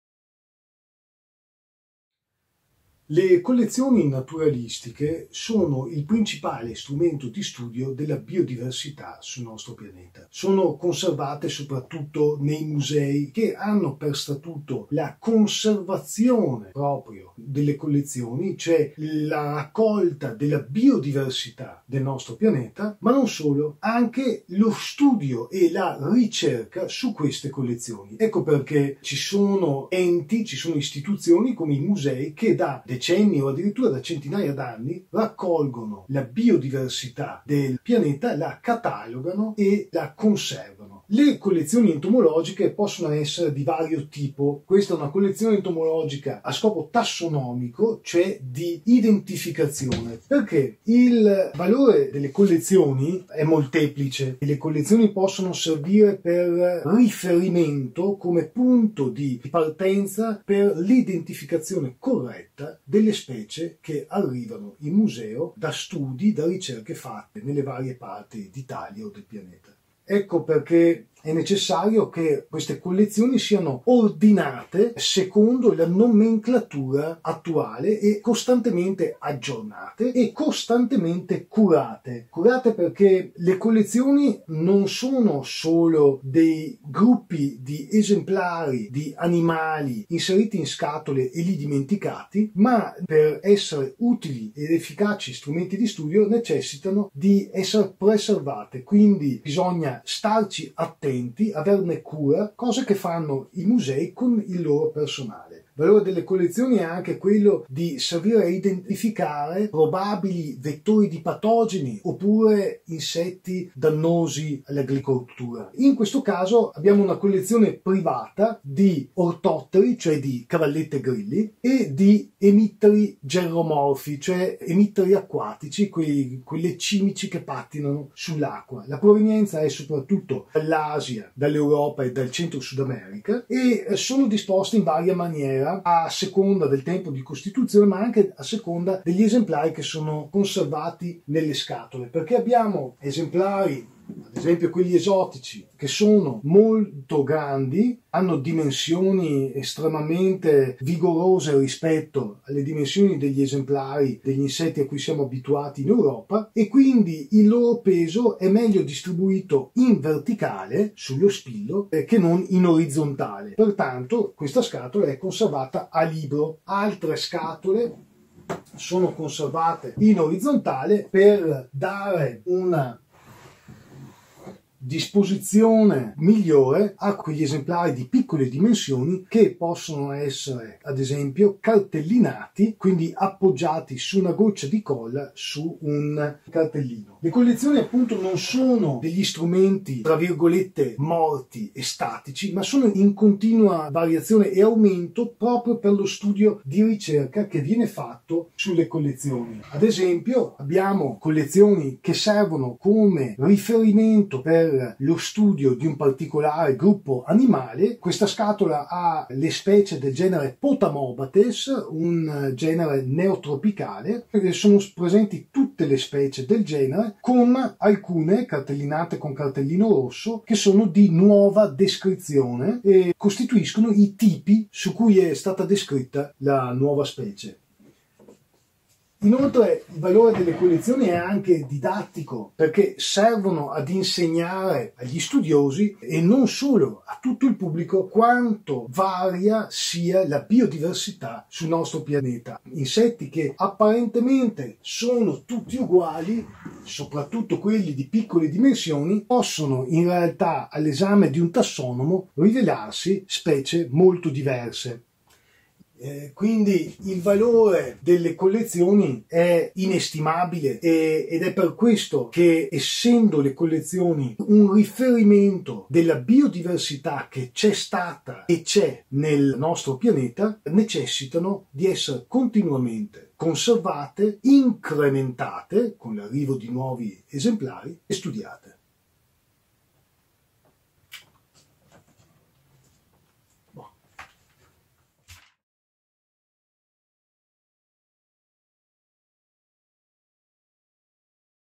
Il tesoro delle collezioni
Filippo Maria Buzzetti, entomologo della Sezione Zoologia della Fondazione Museo Civico, spiega quanto preziose siano le collezioni naturalistiche pubbliche e private per la conoscenza e lo studio della biodiversità mondiale.